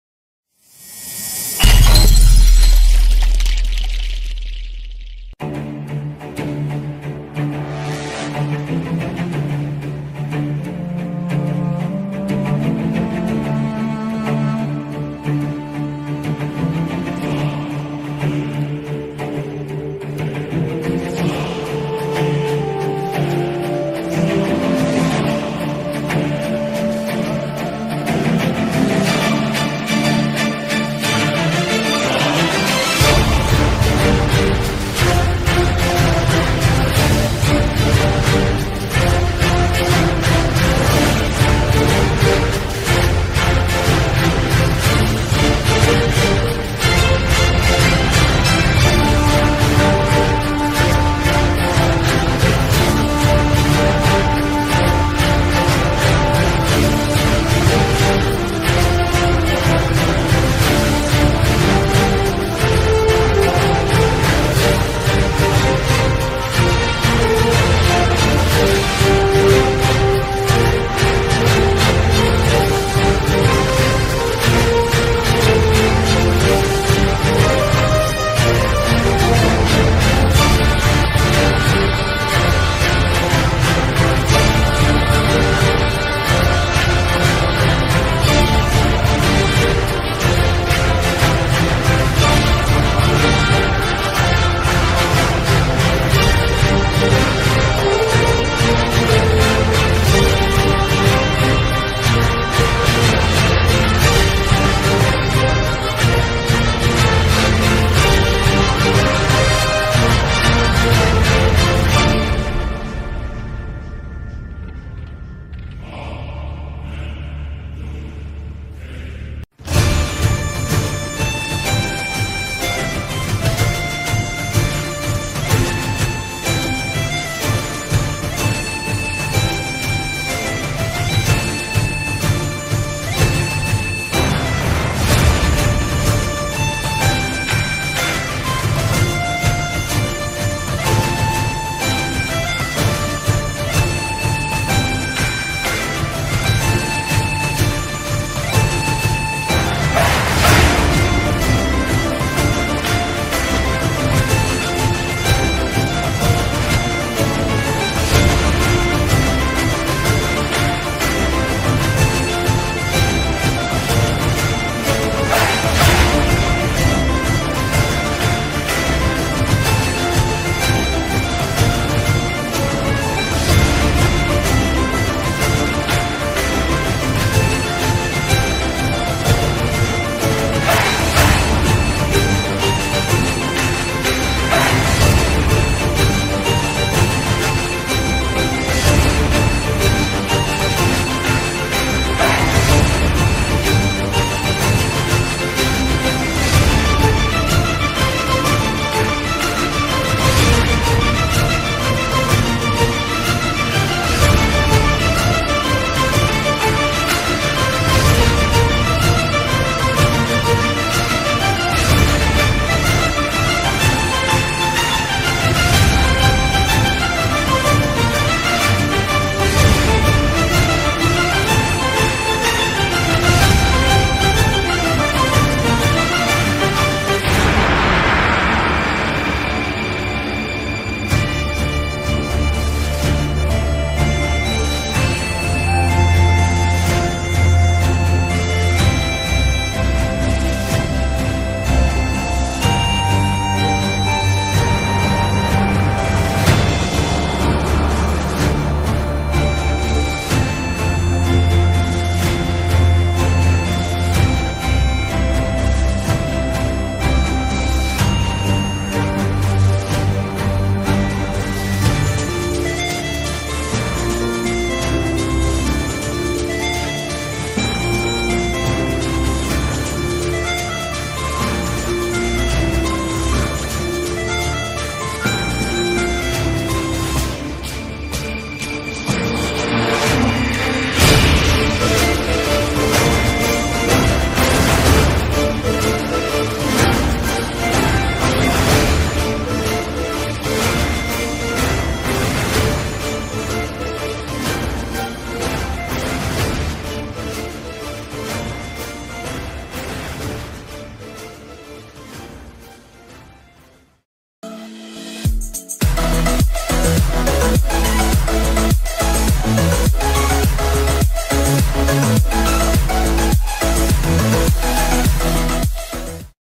How Powerful is Iran in 2021.... Iran Military Power in 2021
Hii Guys today we are know abou Iran military strength in 2021

Please watch till end this video
Thankyou😊😊